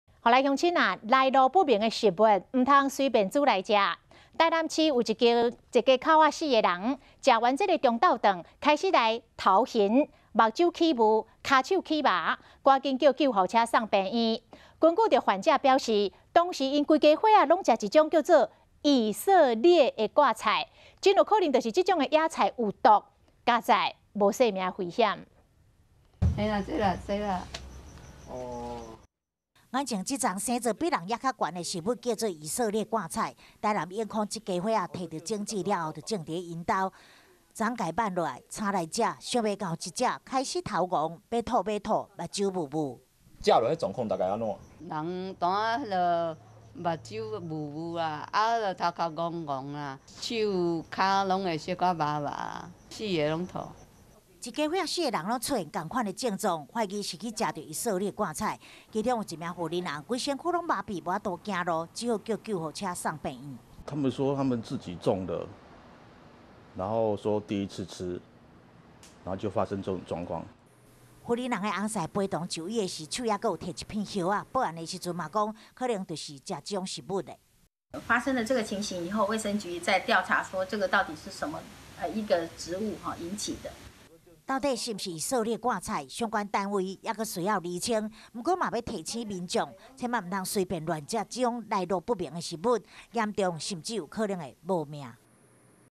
【台語新聞】疑食用自種「以色列芥菜」 一家4口中毒 | 華視新聞 20200312
提醒您千萬不要隨便吃，來路不明的植物，台南市衛生局，昨(11日)接到醫院通報，有一家四口，吃過午餐後就出現嘔吐、全身麻痺的現象，趕緊叫救護車送醫，根據患者表示，當時全家人都吃了一種「以色列芥菜」，相關單位初步懷疑，可能是植物本身的毒性，導致中毒現象，幸好送醫治療後，都沒有生命危險。